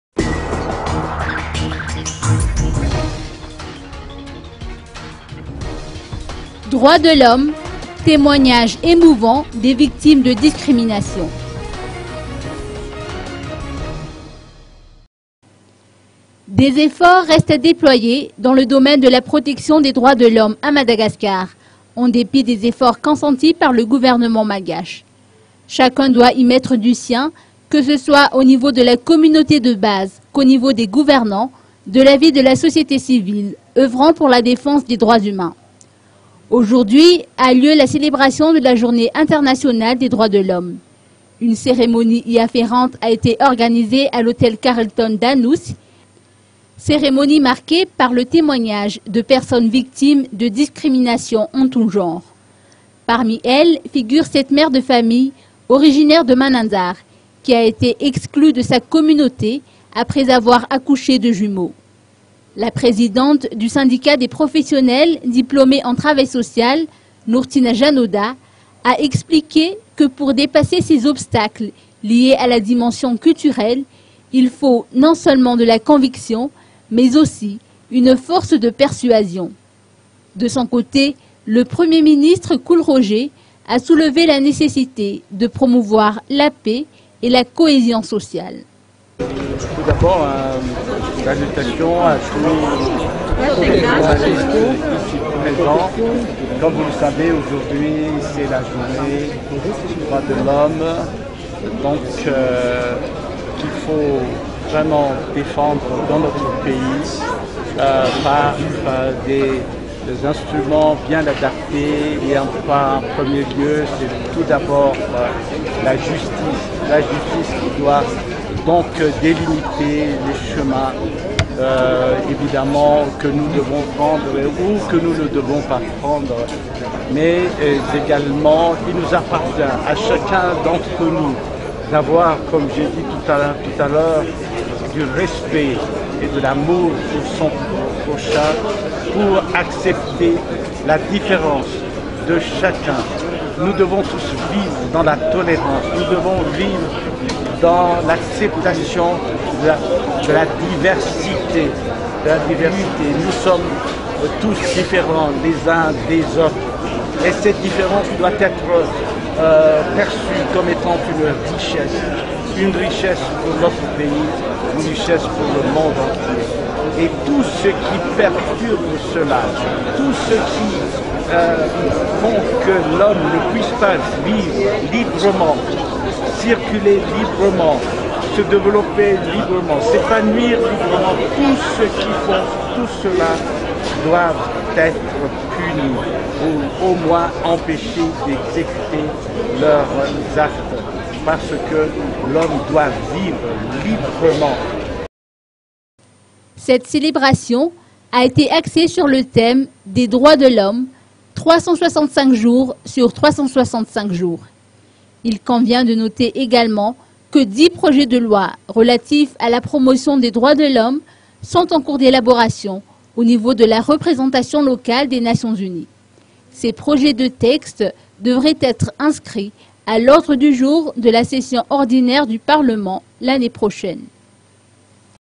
01 DROIT
D e s   e f f o r t s   r e s t e n t   à   d é p l o y e r   d a n s   l e   d o m a i n e   d e   l a   p r o t e c t i o n   d e s   D r o i t s   d e   l ' H o m m e   à   M a d a g a s c a r ,   e n   d é p i t   d e s   e n g a g e m e n t s   c o n s e n t i s   p a r   l e   g o u v e r n e m e n t   m a l g a c h e .